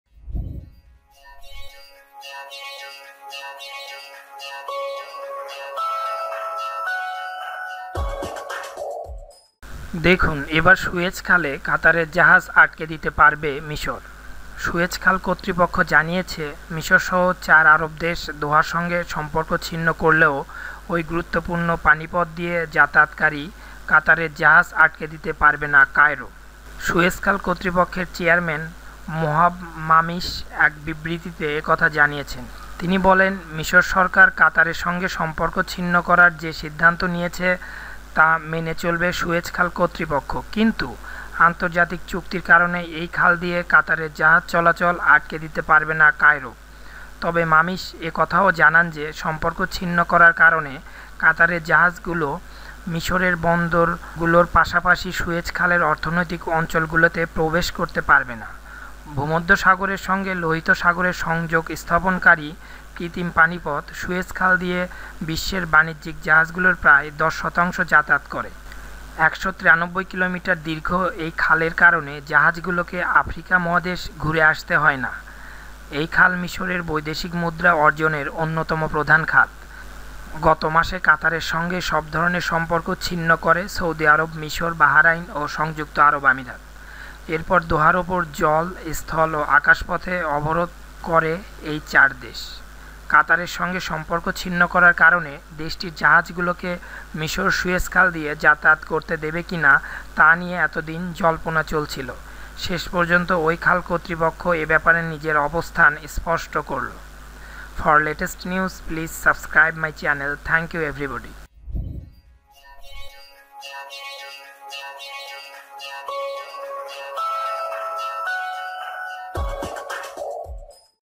দেখুন এবার সুয়েজ খালে কাতারের জাহাজ আটকে দিতে পারবে কি মিসর!!!
সুয়েজ খাল কর্তৃপক্ষ জানিয়েছে, মিশরসহ চার আরব দেশ দোহার সঙ্গে সম্পর্ক ছিন্ন করলেও ওই গুরুত্বপূর্ণ পানিপথ দিয়ে যাতায়াতকারী কাতারের জাহাজ আটকে দিতে পারবে না কায়রো।
সুয়েজ খাল কর্তৃপক্ষের চেয়ারম্যান মোহাব মামিশ এক বিবৃতিতে একথা জানিয়েছেন। তিনি বলেন, মিশর সরকার কাতারের সঙ্গে সম্পর্ক ছিন্ন করার যে সিদ্ধান্ত নিয়েছে তা মেনে চলবে সুয়েজ খাল কর্তৃপক্ষ। কিন্তু আন্তর্জাতিক চুক্তির কারণে এই খাল দিয়ে কাতারের জাহাজ চলাচল আটকে দিতে পারবে না কায়রো।
তবে মামিশ একথাও জানান যে, সম্পর্ক ছিন্ন করার কারণে কাতারের জাহাজগুলো মিশরের বন্দরগুলোর পাশাপাশি সুয়েজ খালের অর্থনৈতিক অঞ্চলগুলোতে প্রবেশ করতে পারবে না।
ভূমধ্যসাগরের সঙ্গে লোহিত সাগরের সংযোগ স্থাপনকারী কৃত্রিম পানিপথ- সুয়েজ খাল দিয়ে বিশ্বের বাণিজ্যিক জাহাজগুলোর প্রায় ১০ শতাংশ যাতায়াত করে। ১৯৩ কিলোমিটার দীর্ঘ এই খালের কারণে জাহাজগুলোকে আফ্রিকা মহাদেশ ঘুরে আসতে হয় না। এই খাল মিশরের বৈদেশিক মুদ্রা অর্জনের অন্যতম প্রধান খাত।
গতমাসে কাতারের সঙ্গে সব ধরনের সম্পর্ক ছিন্ন করে সৌদি আরব, মিশর, বাহরাইন ও সংযুক্ত আরব আমিরাত। এরপর দোহার ওপর জল, স্থল ও আকাশপথে অবরোধ আরোপ করে ওই চার দেশ। কাতারের সঙ্গে সম্পর্ক ছিন্ন করার কারণে দেশটির জাহাজগুলোকে মিশর সুয়েজ খাল দিয়ে যাতায়াত করতে দেবে কিনা তা নিয়ে এতদিন জল্পনা চলছিল। শেষ পর্যন্ত ওই খাল কর্তৃপক্ষ এ ব্যাপারে নিজের অবস্থান স্পষ্ট করল।